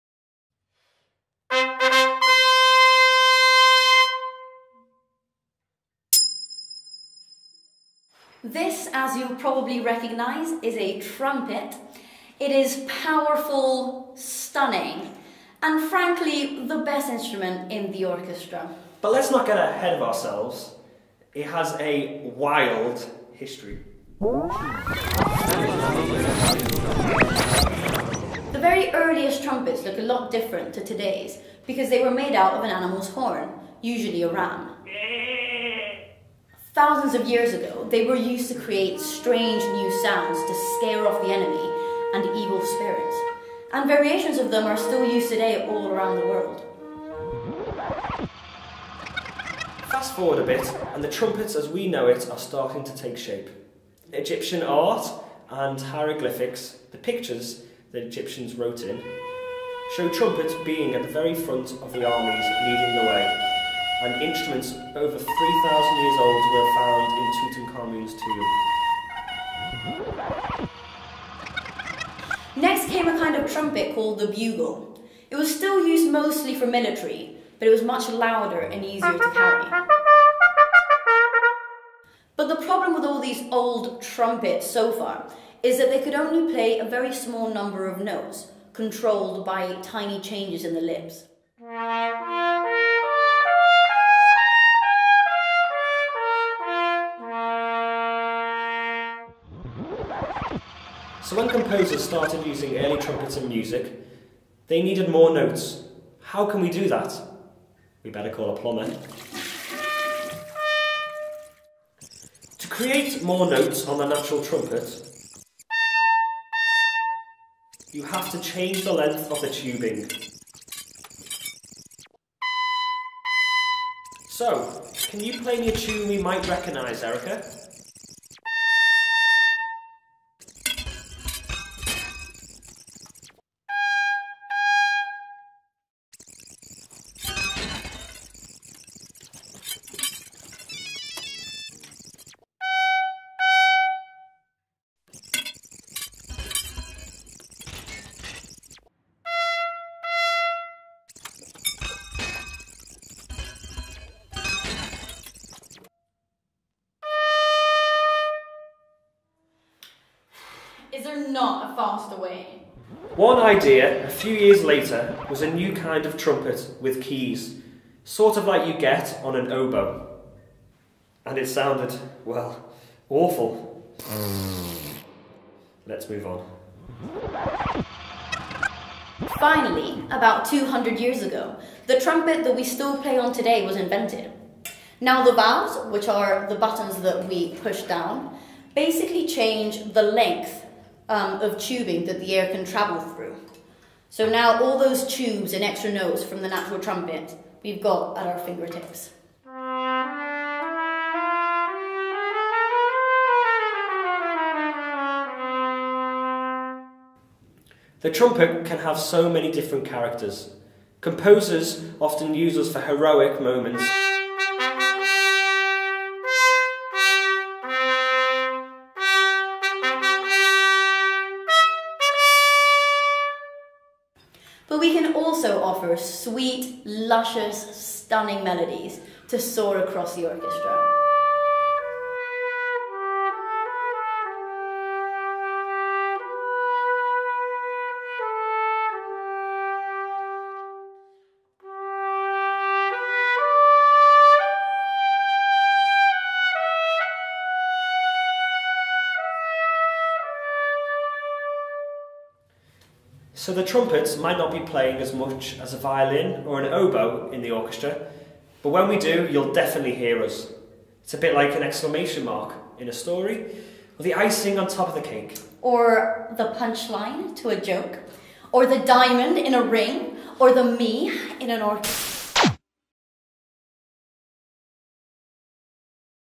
Meet the Trumpet with Peter and Erika Southbank Sinfonia's Family Concert
Thanks Erica and Peter, for allowing me to share this. For those of you with children at home it might provide 5 minutes of amusement, and add a bit to their musical education??